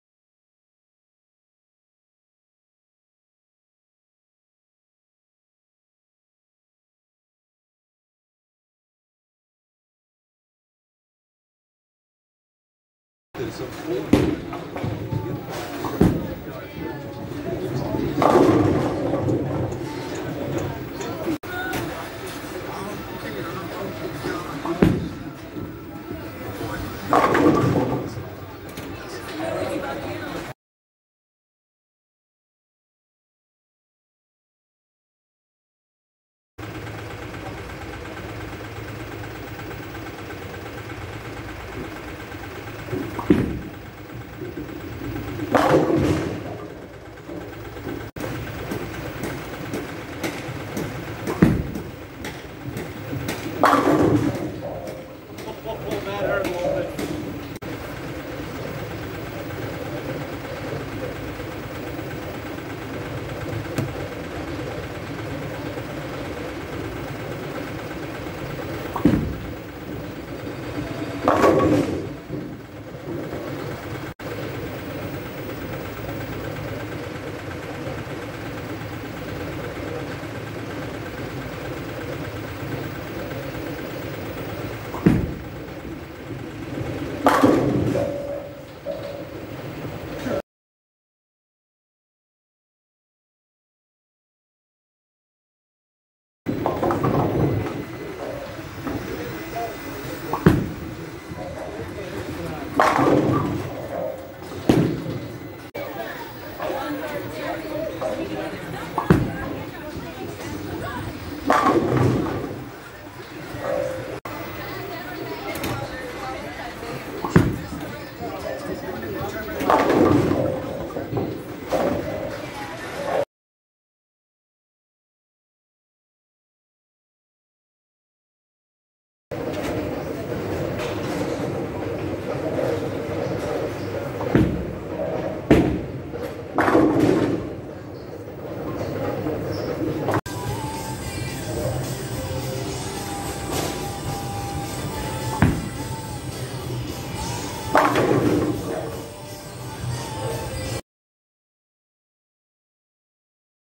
6/4/2021 Bowling Ball Arsenal Update
Hey everyone! I know it's nothing big, but here is a video showing my current arsenal of bowling balls. This will soon expand as I gain more contract balls from California Bowling, and I will shoot new videos when they come in! New videos will be uploaded two-three times every week as per my new schedule!